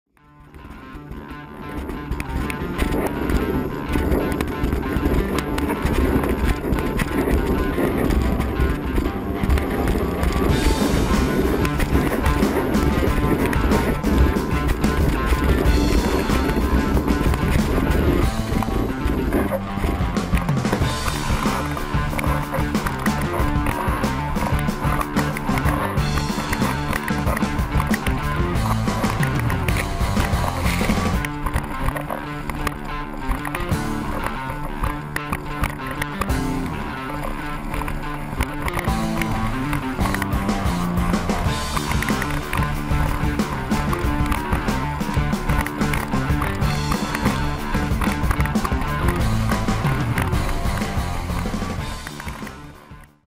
Dark Horse 132 Planning
J’ai sortie ma Future Fly Board Dark Horse 132 avec une vieille Gaastra Matrix 8.5 que j'aime bien.  L'aileron est un Z CF 44 S- un vent entre 8 et 12 noeuds.

I took out my Future Fly Board Dark Horse 132 with an old Gaastra Matrix 8.5 that I like.  The fin is a Z CF 44 S-  The wind is between 8 and 12 knots.